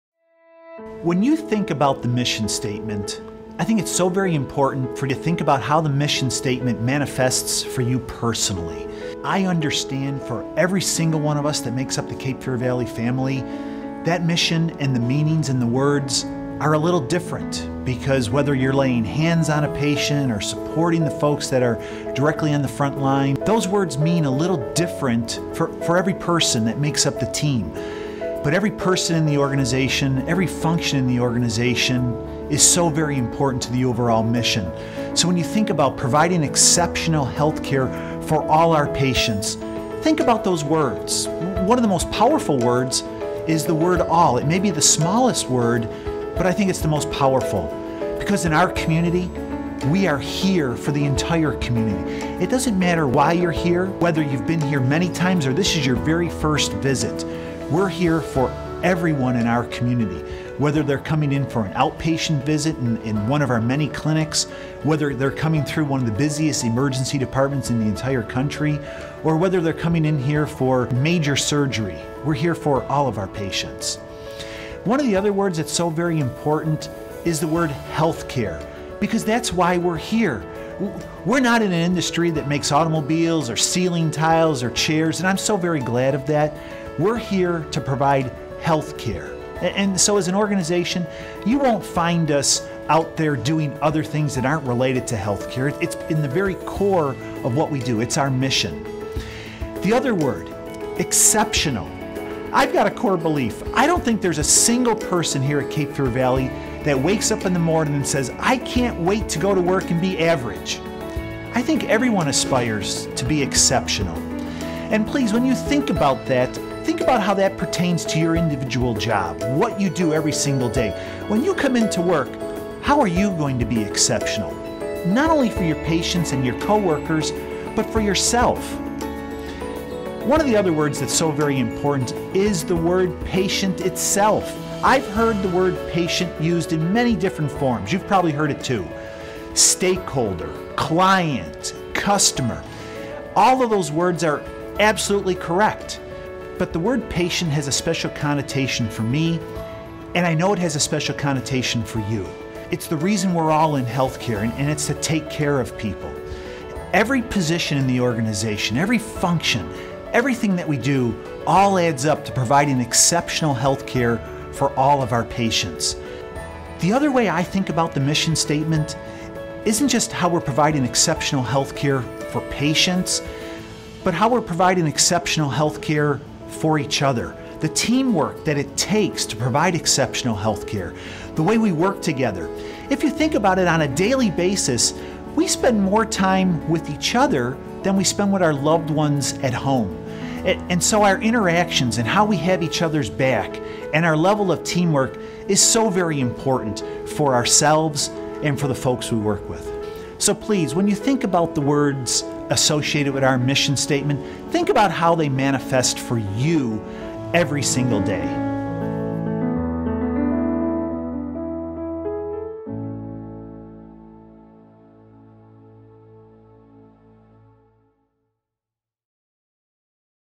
CFV MISSION STATEMENT
Cape Fear Valley Health CEO, Michael Nagowski, speaks on how he feels the mission to provide exceptional healthcare relates to each individual employee.